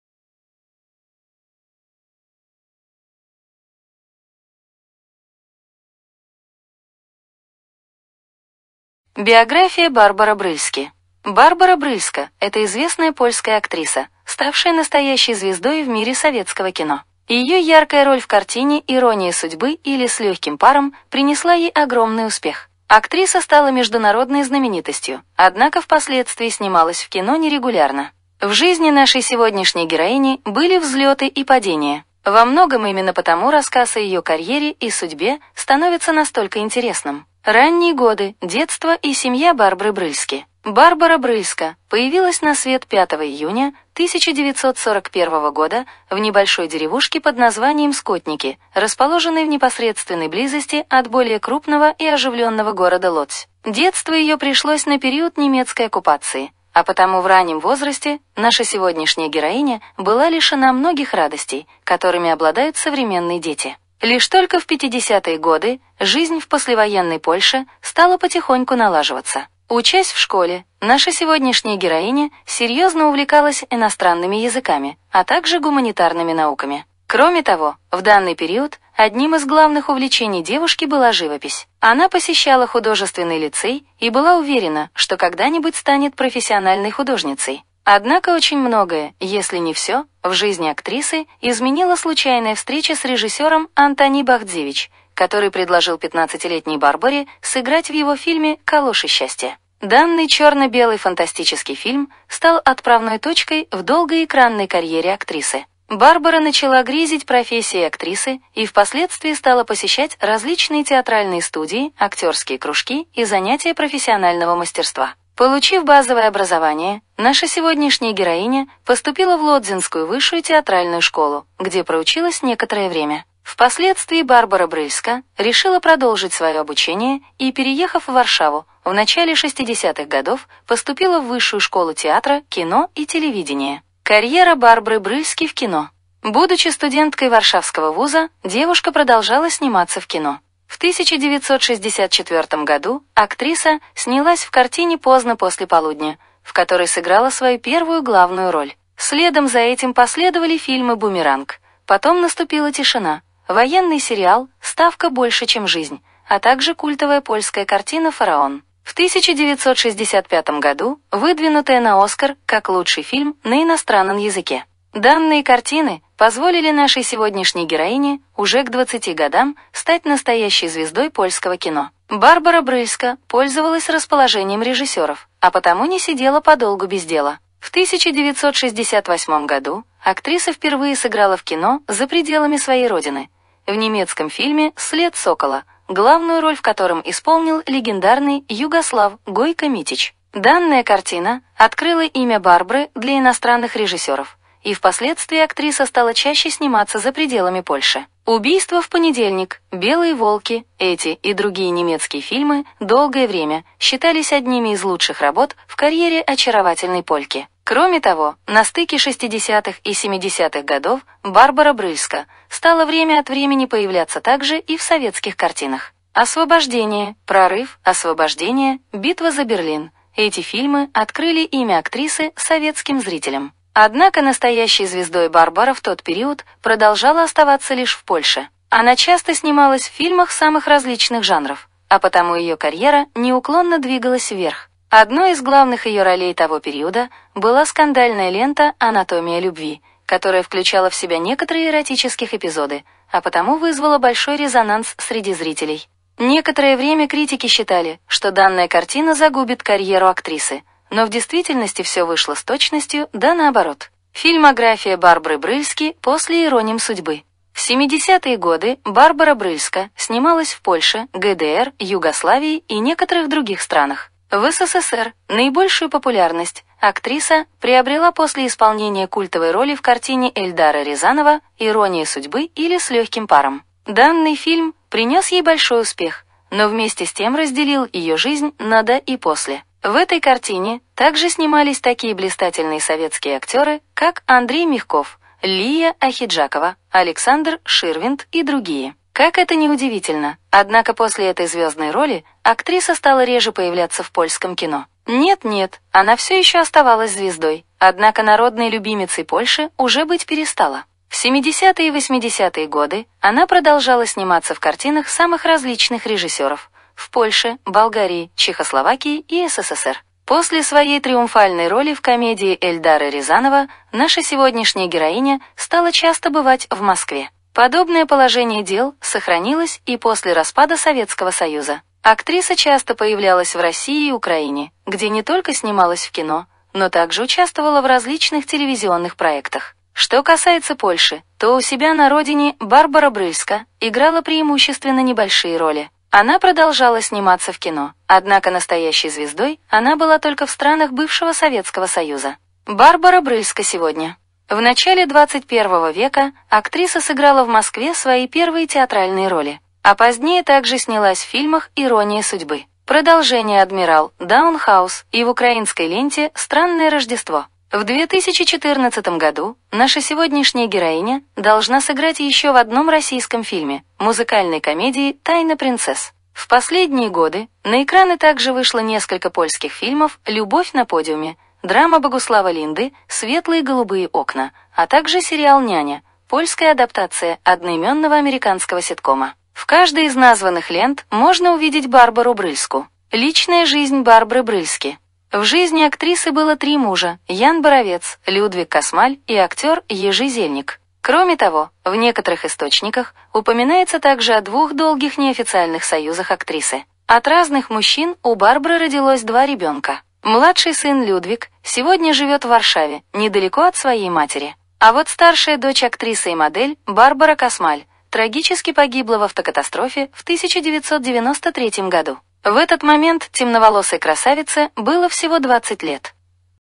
Барбара Брыльска
История жизни и биография Барбары Брыльки

0:00 intro
0:38 дата и место рождения
1:10 школьные годы
1:40 актерская карьера
2:25 первый фильм
4:15 фильмография